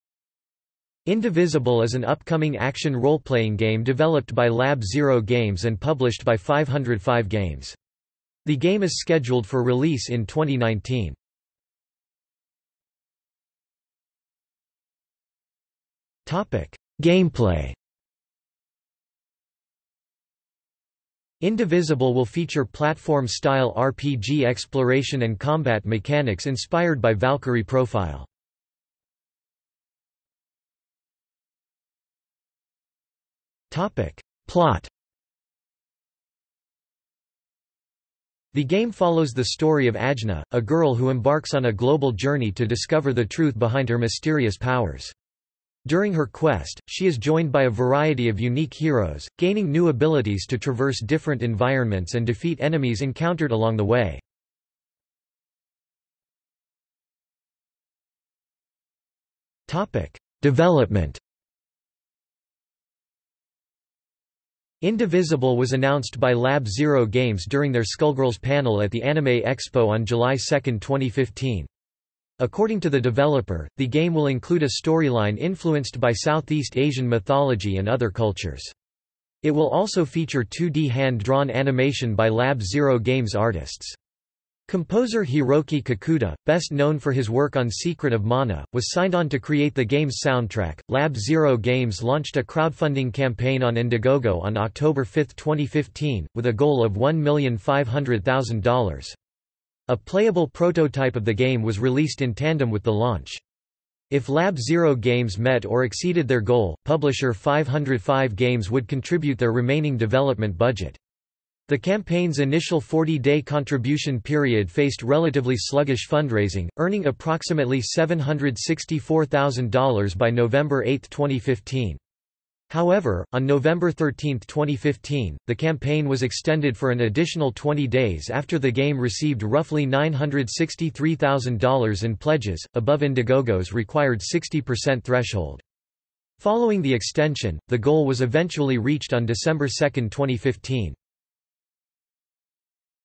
Indivisible (video game) | Wikipedia audio article
00:00:12 1 Gameplay
00:00:30 2 Plot
00:00:56 3 Development

- Socrates

SUMMARY
Indivisible is an upcoming action role-playing game developed by Lab Zero Games and published by 505 Games. The game is scheduled for release in 2019.